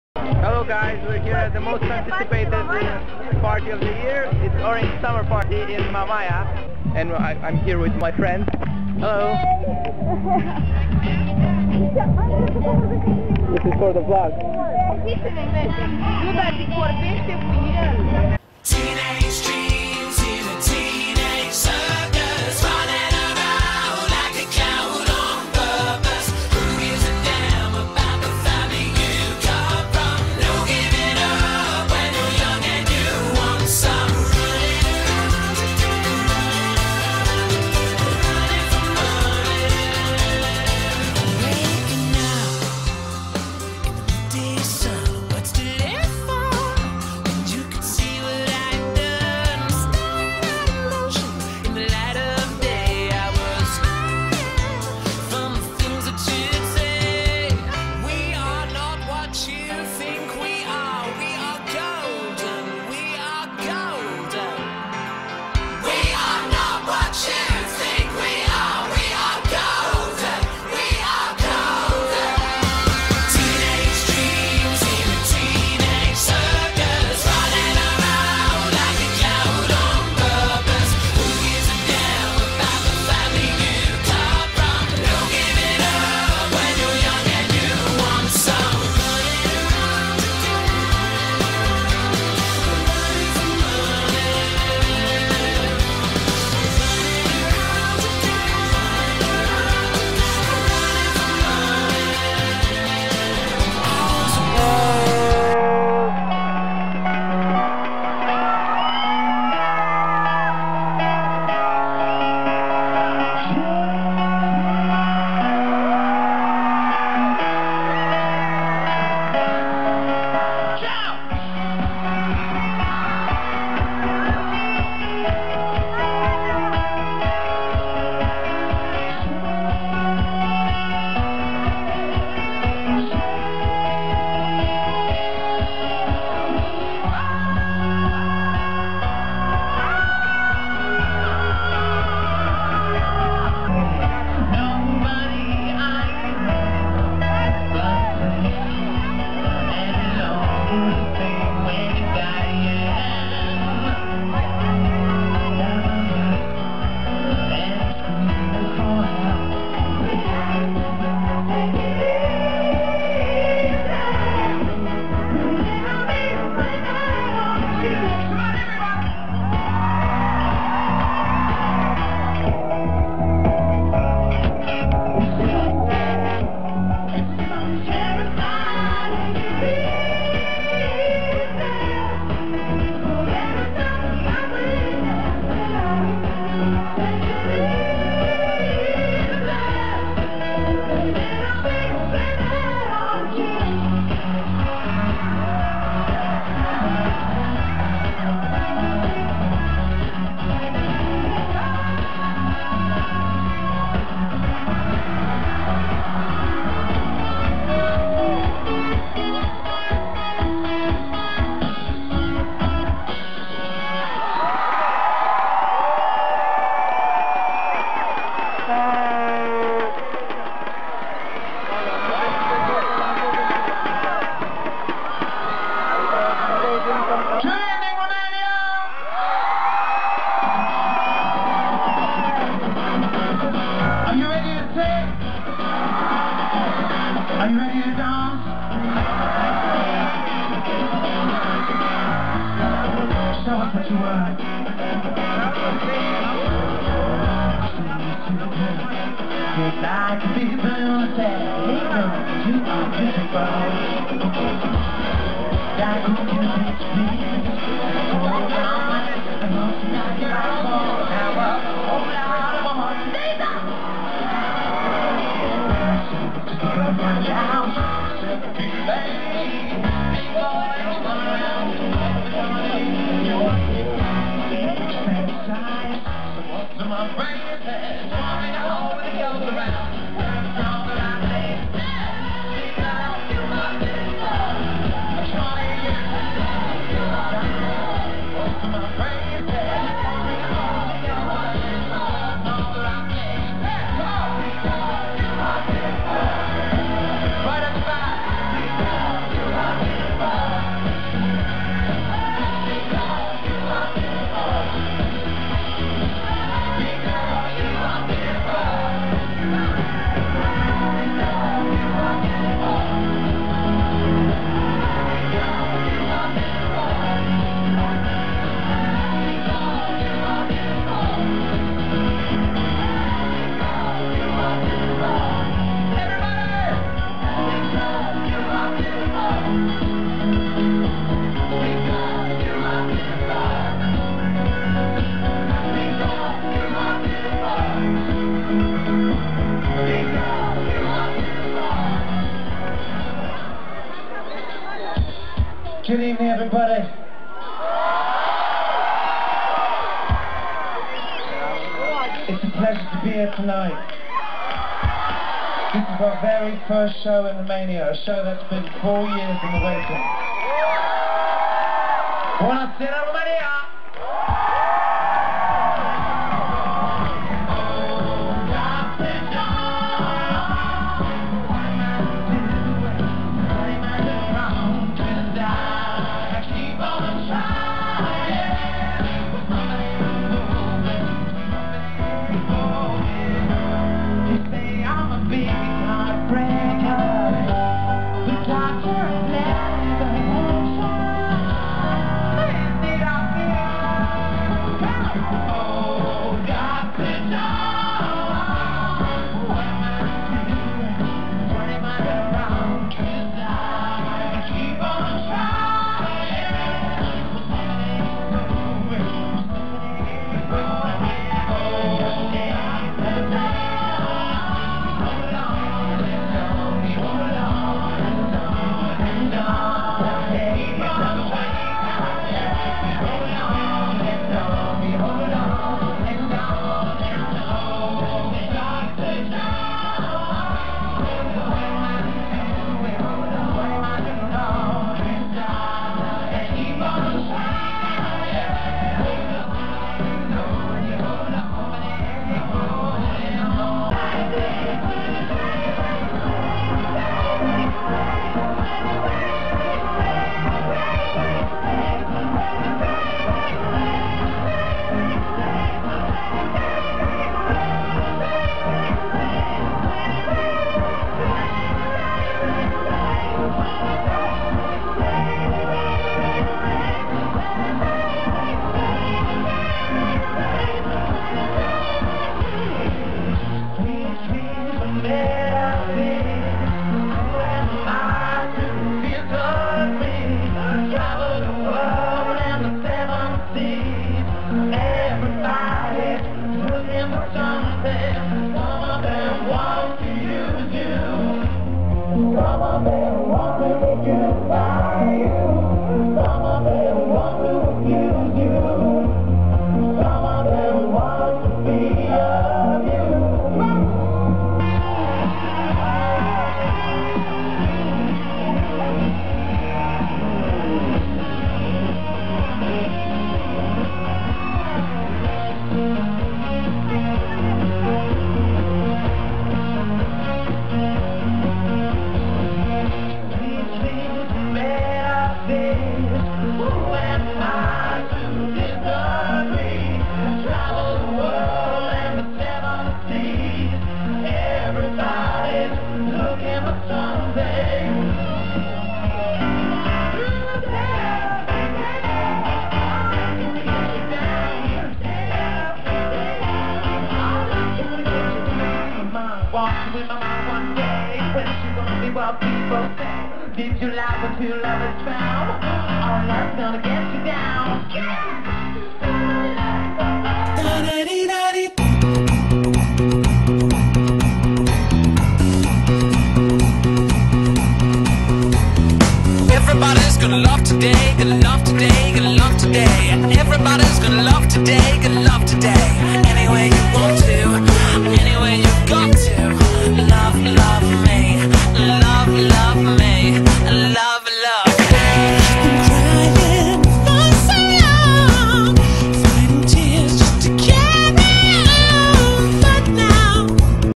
Mamaia + Mika concert (Orange summer party) part 2
Mika concert on the H2O beach in Mamaia!!! (Orange summer party)